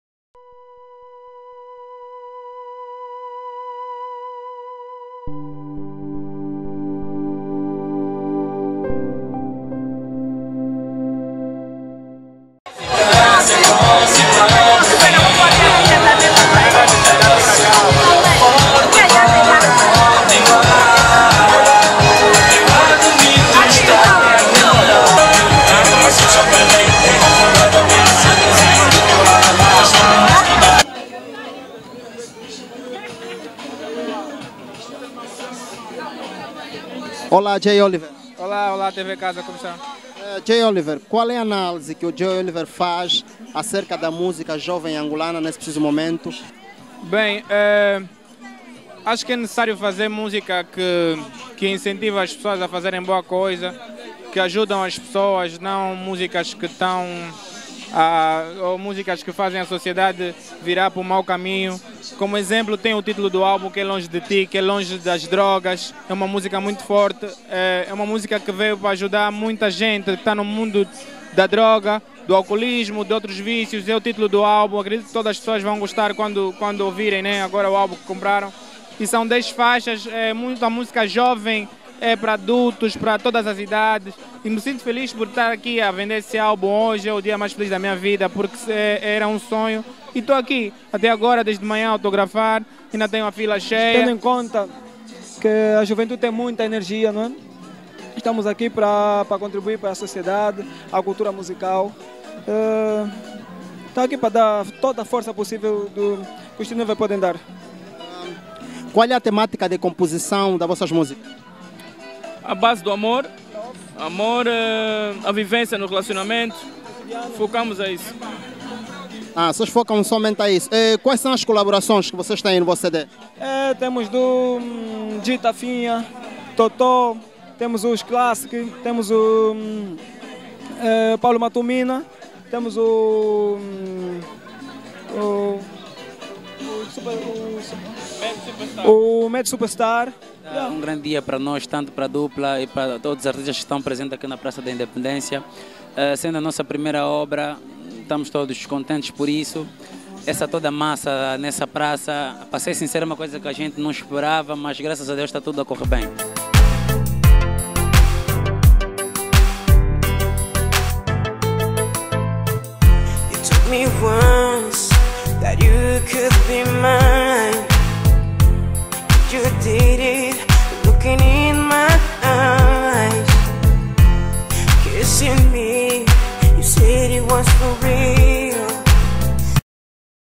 JAY OLIVER, TEENOVER E LIONSEL SATISFEITOS NA PRAÇA DA INDEPENDENCIA
A praça da independencia esteve cheia de gente interessada em comprar discos dos artistas Jay Oliver, Teenover, Wara e Lionsel